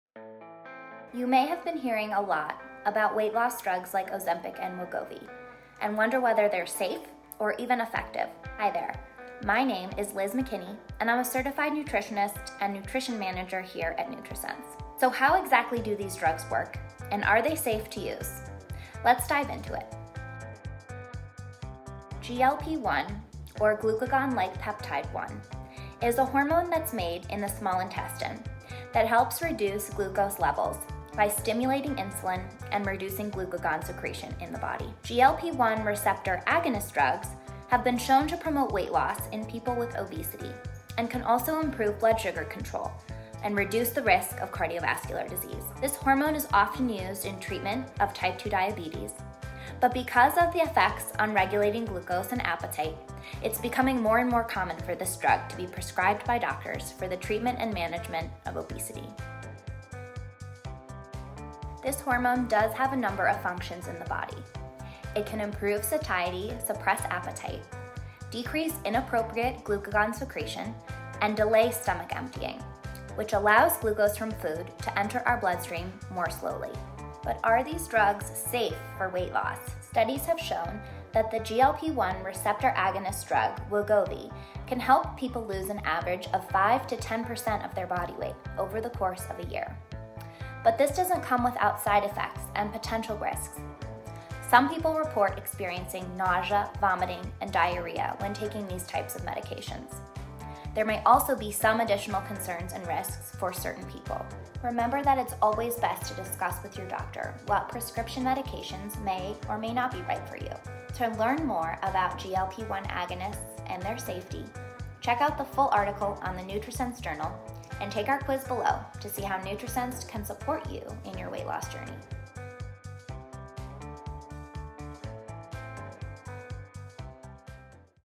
GLP-1 for Weight Loss: Is it Safe?
Wondering if GLP-1 drugs like Ozempic or Wegovy are safe or effective for weight loss? Certified Nutrisense nutritionist Liz McKinney, MS, CNS, LDN shares the facts about GLP-1 receptor agonists, what function they have in the body, and what the research has to say about GLP-1 drugs and their potential side effects here.

To learn more about GLP-1 agonists and their safety, read the full article

Video Chapters:
0:00:  Intro to Weight Loss Drugs
0:21: What is GLP-1?
0:38: When Are GLP-1 Agonist Drugs Used
0:59: Functions of GLP-1 Agonists in the Body
1:20 Are GLP-1 Agonists Safe?
1:34: Potential Side Effects
1:53: Where to Learn More